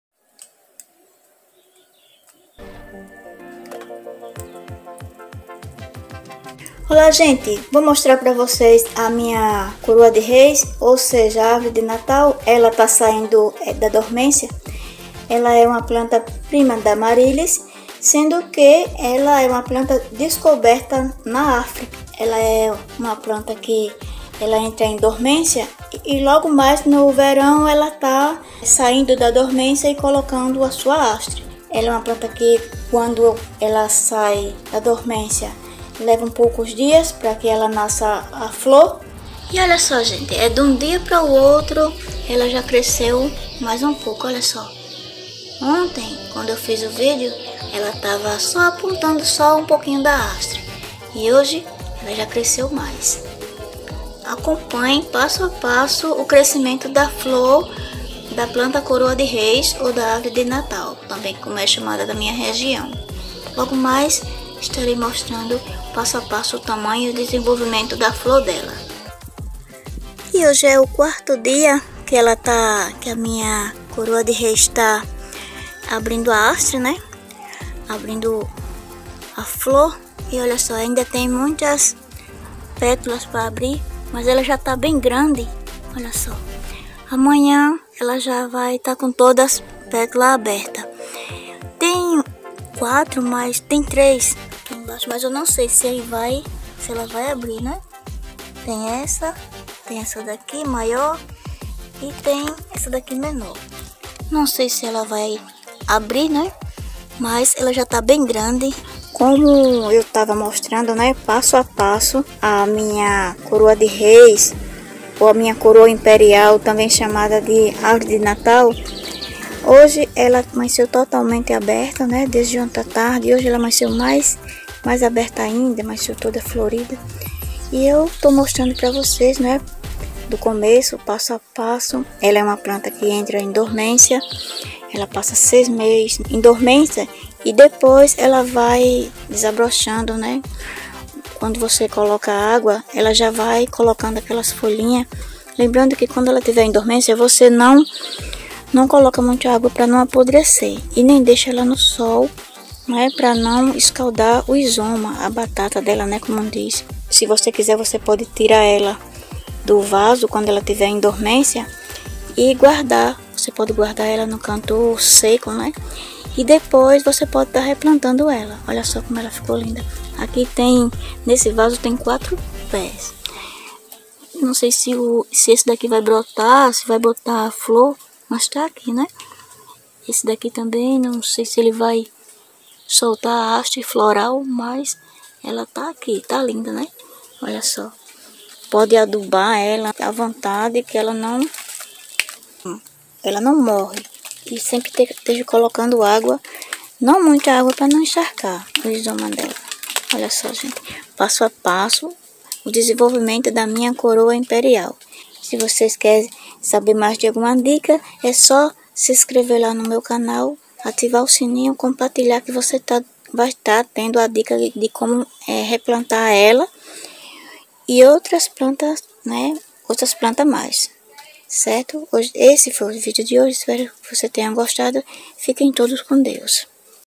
Planta Coroa Imperial Como Cultivar Estrela de Natal
Como Plantar Coroa Imperial também Conhecida como Flor de Natal fazer mudas da coroa imperial
coroa imperial cuidados //
como fazer mudas da coroa imperial --
planta estrela de natal ||
planta flor de natal